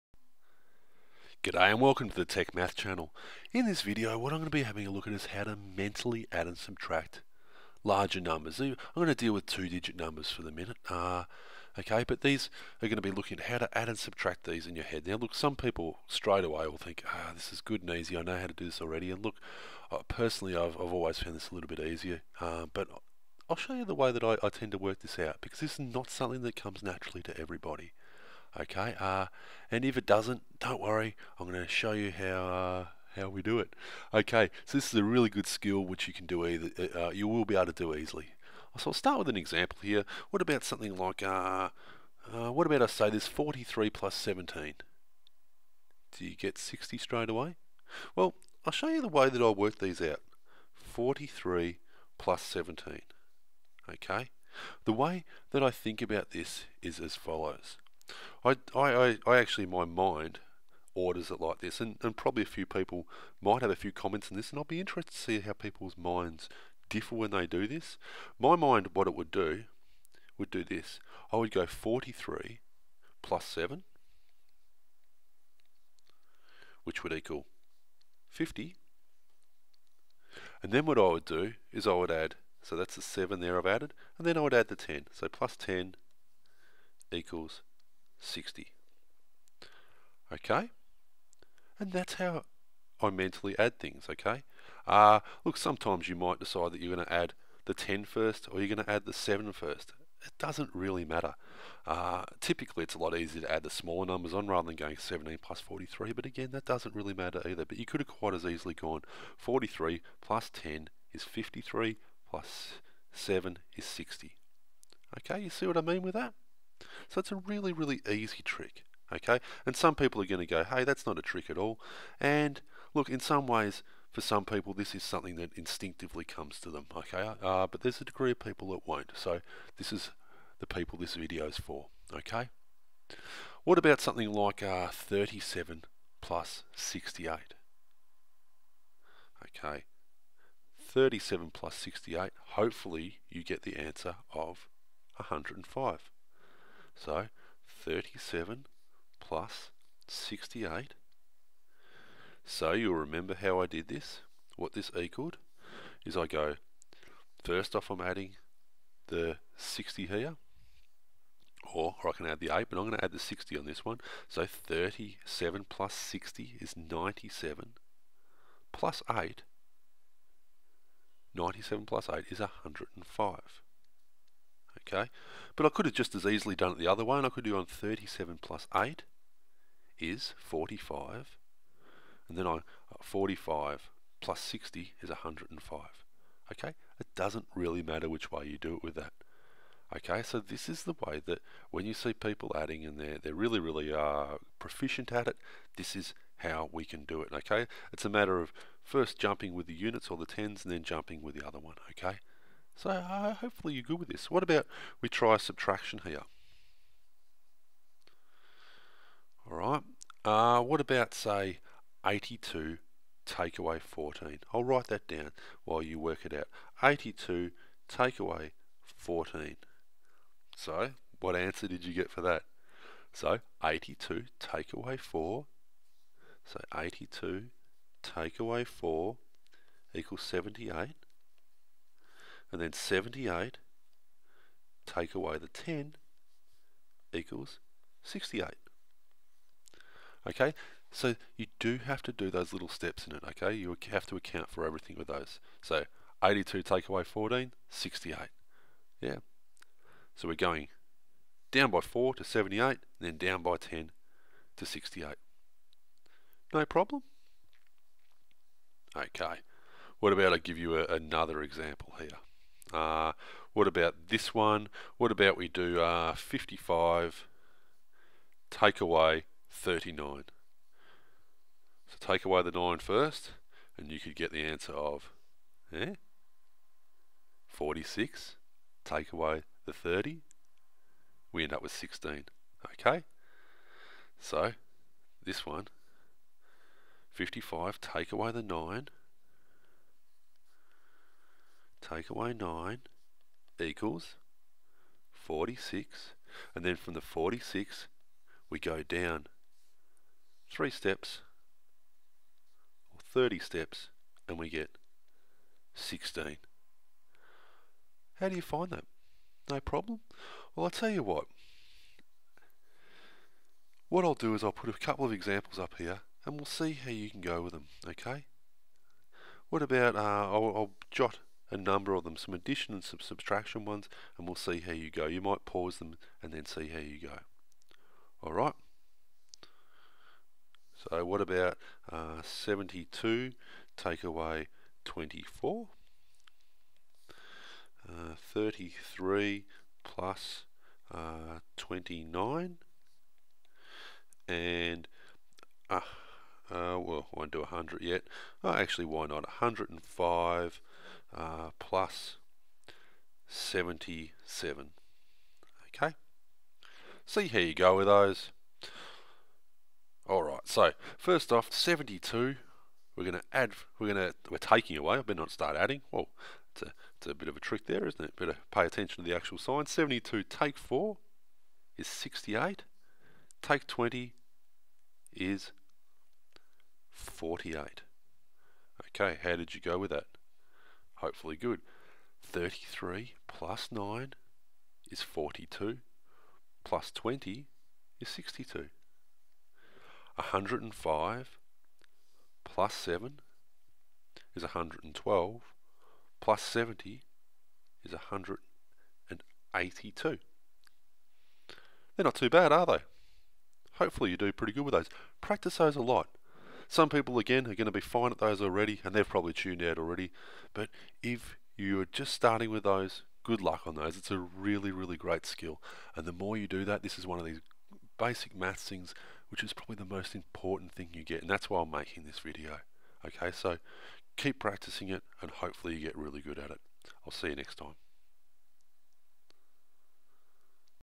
Mental Math Tricks - Addition and Subtraction in your head!
With this mental math trick you will beable to add and subtract in your head rapidly - a critical skill in everyday math.  In this video we look at tips on how to do basic addition and subtraction instantly without using a calculator.  Fast math is good math!